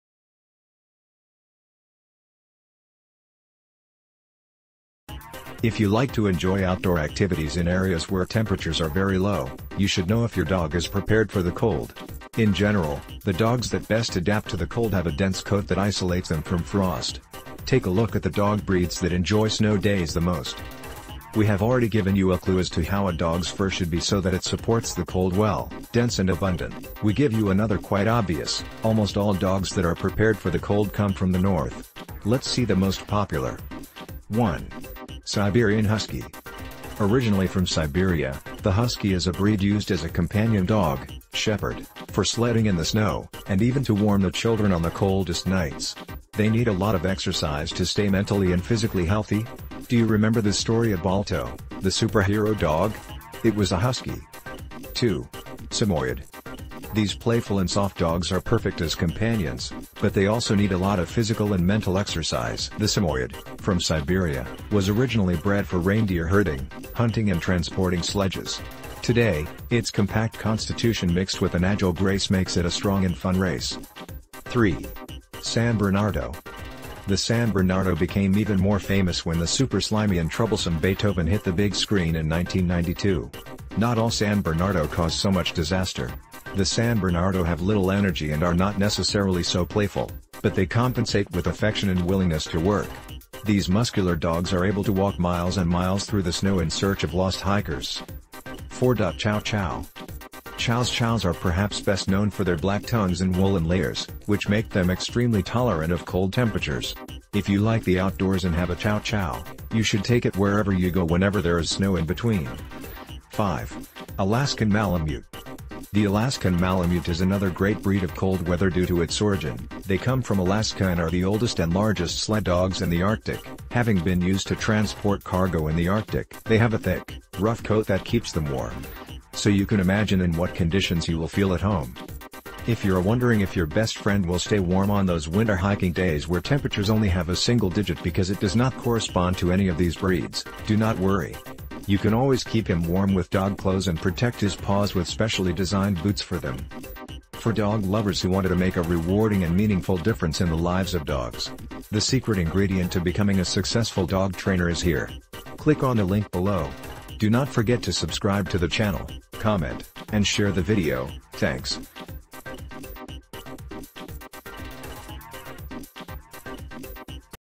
TOP 5 Dog Breeds That Best Adapt In Cold Climates
If you like to enjoy outdoor activities in areas where temperatures are very low, you should know if your dog is prepared for the cold. In general, the dogs that best adapt to the cold have a dense coat that isolates them from frost. Take a look at the dog breeds that enjoy snow days the most.

We have already given you a clue as to how a dog's fur should be so that it supports the cold well (dense and abundant), we give you another quite obvious: almost all dogs that are prepared for the cold come from the north. Let's see the most popular. SEE THE VIDEO...

1. Siberian Husky.
2. Samoyed.
3. San Bernardo.
4.Chow Chow.
5. Alaskan Malamute.

The Alaskan Malamute is another great breed of cold weather due to its origin: they come from Alaska and are the oldest and largest sled dogs in the Arctic, having been used to transport cargo in the Arctic. They have a thick, rough coat that keeps them warm. So you can imagine in what conditions you will feel at home.

If you're wondering if your best friend will stay warm on those winter hiking days where temperatures only have a single digit because it does not correspond to any of these breeds, do not worry. You can always keep him warm with dog clothes and protect his paws with specially designed boots for them.